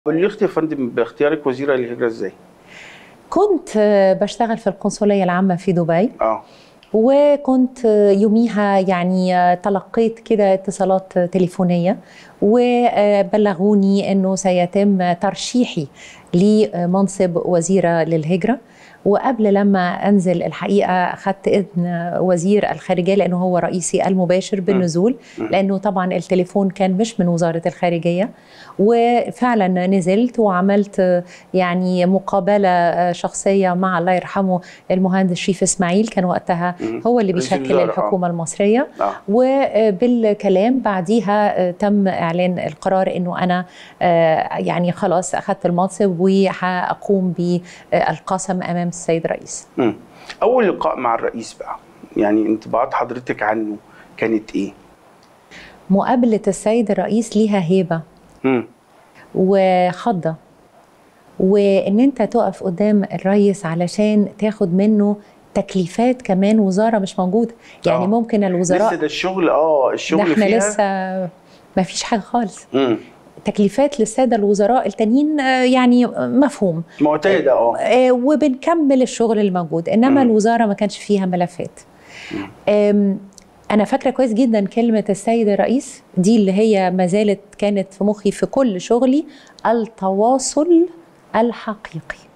الشاهد | السفيرة نبيلة مكرم: تلقيت خبر تولي حقيبة الهجرة وأنا في القنصلية العامة بدبي
السفيرة نبيلة مكرم وزيرة الدولة لشؤون الهجرة السابقة:
تلقيت خبر تولي حقيبة الهجرة وأنا في القنصلية العامة بدبي

15 نوفمبر 2023